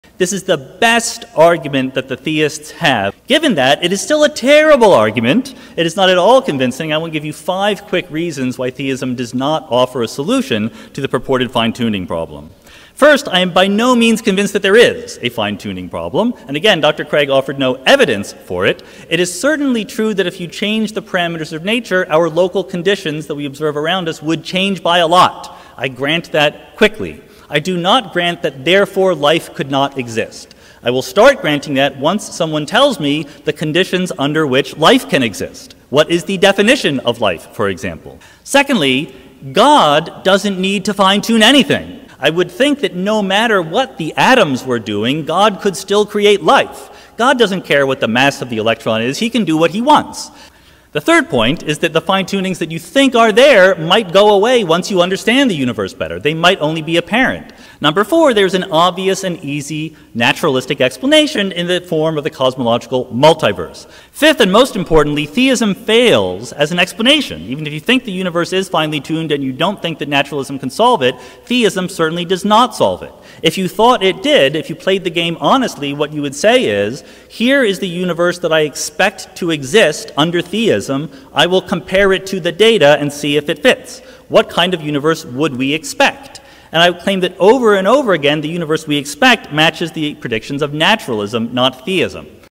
Sean Carroll on the Fine Tuning Argument
This is a clip I made for my critique video "20 Reasons You can be Confident Christianity is True", but I thought this would be a good stand alone video as well.